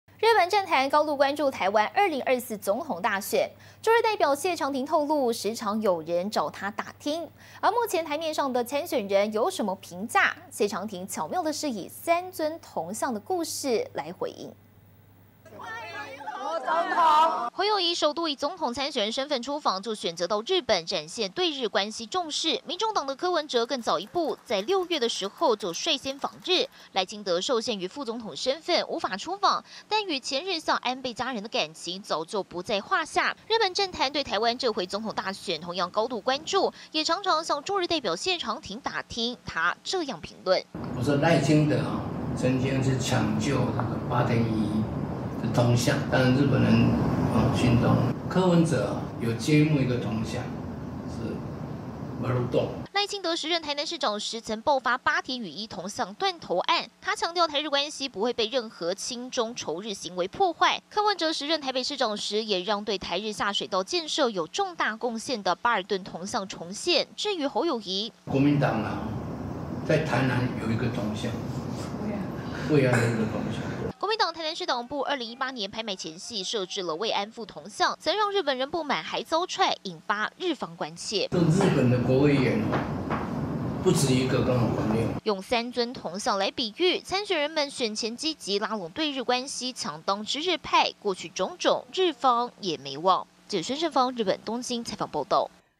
賴清德修復「八田與一」 柯P為「巴爾頓」揭幕
日本政壇高度關注台灣2024總統大選，駐日代表謝長廷透露，時常有人找他打聽，對目前檯面上的參選人有什麼評價？謝長廷巧妙以「3尊銅像」的故事回應，像賴清德曾參與搶救「嘉南大圳之父」八田與一銅像的修復，柯文哲曾為日本下水道之父「巴爾頓」銅像揭幕，而侯友宜所屬的國民黨，則在台南設置「慰安婦銅像」。